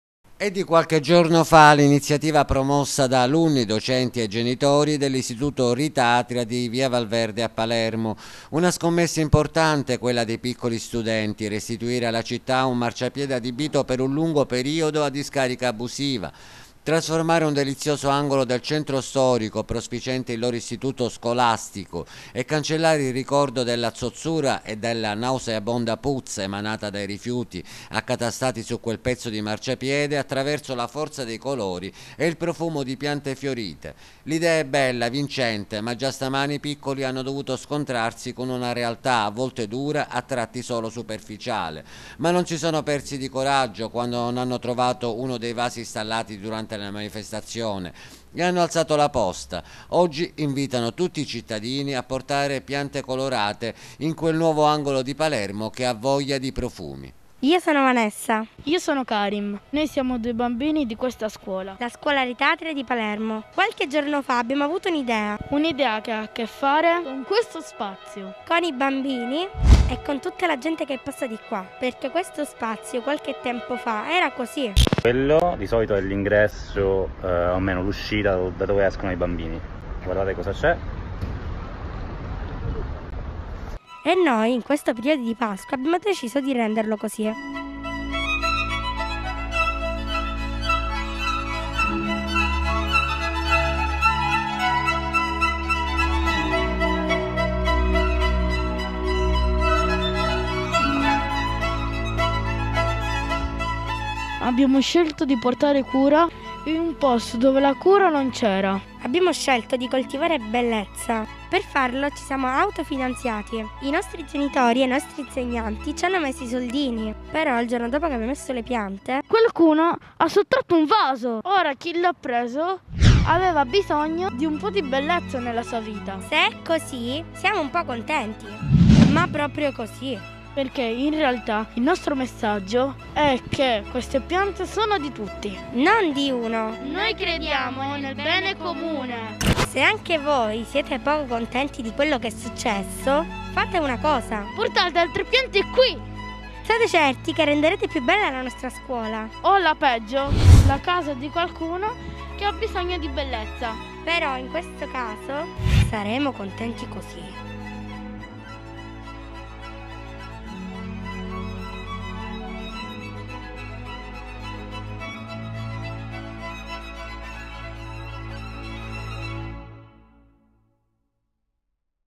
Palermo. Tutti insieme con gli alunni del Rita Atria
Sono passati pochi giorni dall’iniziativa degli alunni della scuola Rita Atria a Palermo, che hanno restituito alla città un marciapiede adibito a discarica abusiva, adornandolo con coloratissime piante fiorite ma già è scomparso un vaso e allora i bambini rilanciano e invitano i palermitani a portare piante in quel pezzo di città allungo dimenticato.